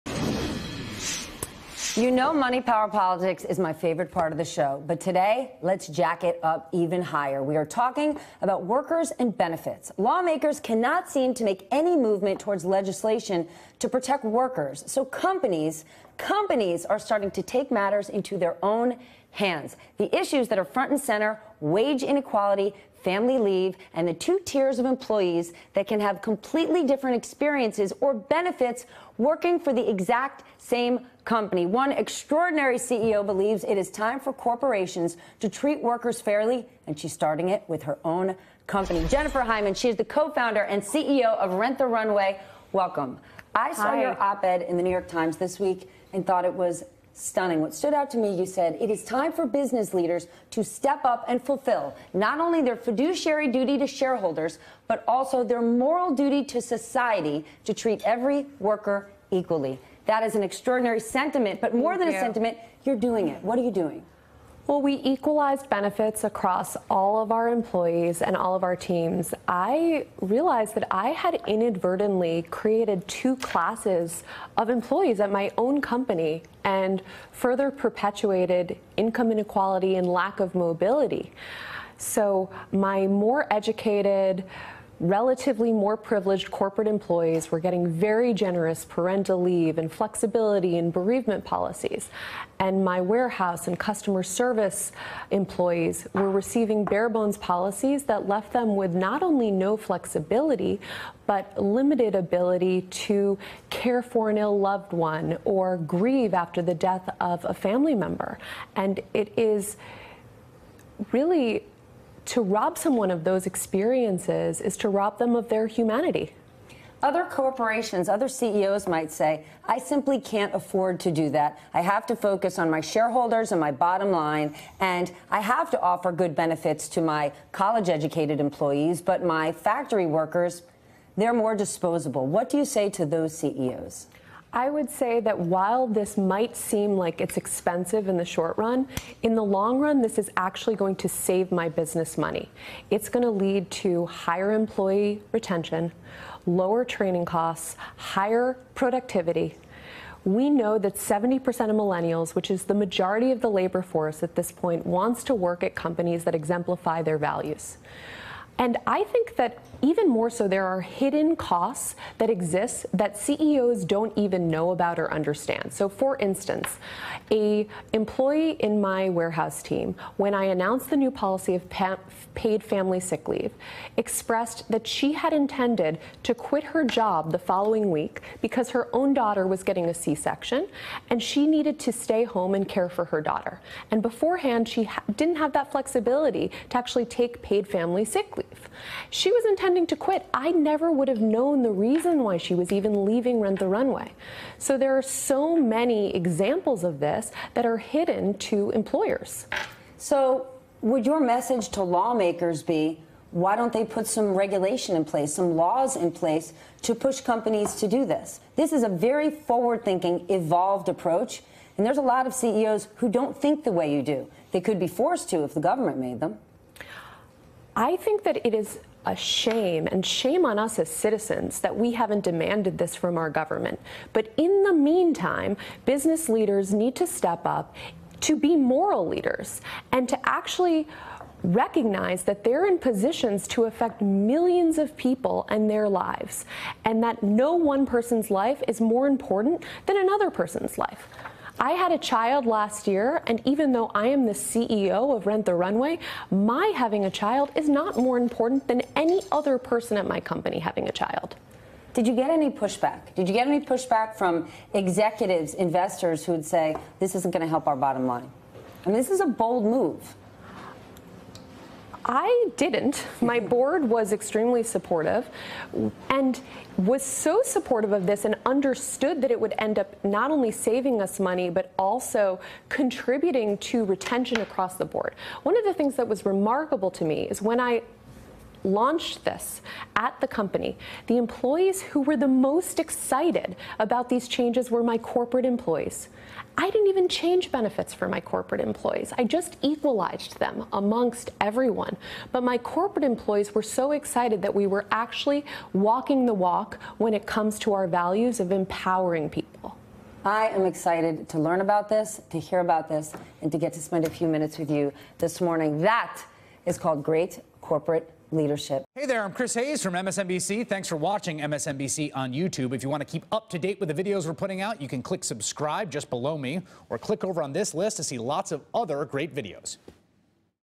Rent The Runway CEO Jennifer Hyman: Equalizing Benefits Will ‘Save My Business Money’ | MSNBC
Rent the Runway CEO Jennifer Hyman believes it’s time for corporations to treat workers fairly, starting with her own company. Money Power Politics: Lawmakers can't seem to make any movement towards legislation to protect workers, so companies are taking matters into their own hands.